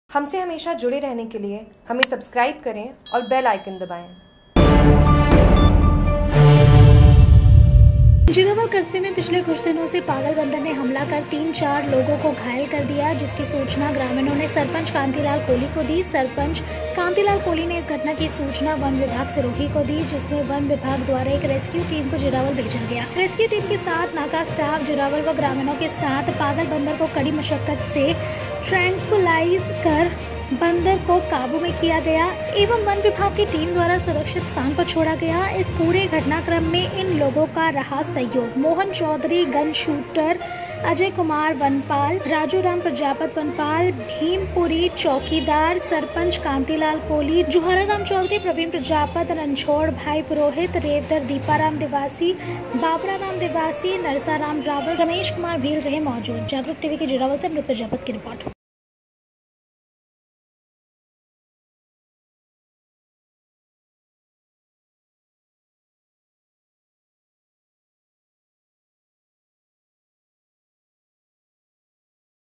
जीरावल : बंदर के हमले में चार लोग घायल
जीरावल कस्बे में पिछले कुछ दिनों से  पागल बंदर ने हमला कर  तीन  चार लोगों को घायल कर दिया ।जिसकी सूचना ग्रामीणों ने सरपंच कांतिलाल कोली को दी।  सरपंच कांतिलाल कोली ने इस घटना की सूचना वन विभाग सिरोही को दी। जिसमें वन विभाग द्वारा एक  रेस्क्यू टीम को जीरावल भेजा गया । रेस्क्यू टीम के साथ नाका स्टाफ जीरावल व ग्रामीणों के साथ पागल बंदर को कड़ी मशक्कत से ट्रेंकुलाइज कर बंदर को काबू में किया गया एवं वन विभाग की टीम द्वारा सुरक्षित स्थान पर छोड़ा गया। इस पूरे घटनाक्रम में इन लोगों का रहा सहयोग मोहन चौधरी गन शूटर, अजय कुमार वनपाल , राजूराम प्रजापत वनपाल, भीमपुरी चौकीदार, सरपंच कांतिलाल कोली, जुहारा राम चौधरी, प्रवीण प्रजापत रणछोड़ भाई पुरोहित रेवदर दीपाराम देवासी, बाबरा राम देवासी, नरसाराम रावल रमेश कुमार भील,